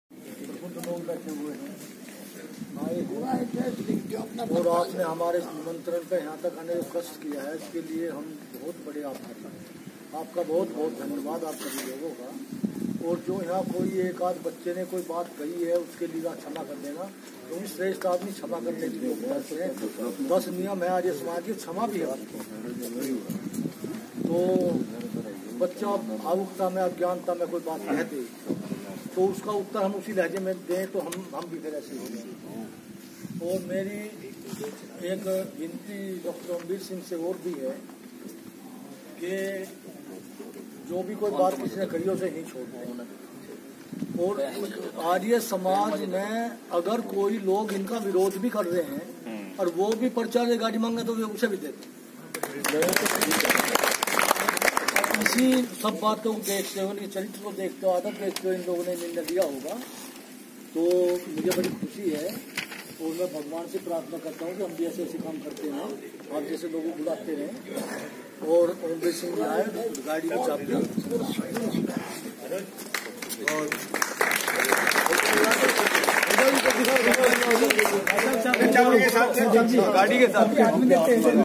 12 July 2022
🙏🔥 श्री ओमवीर आर्य ने आर्य समाज के प्रचार हेतु बागपत जिले में डा. ओमवीर सिंह 🔥आर्य समाज बड़ौत 🔥को एक न्यू गाड़ी भेट की।🔥🙏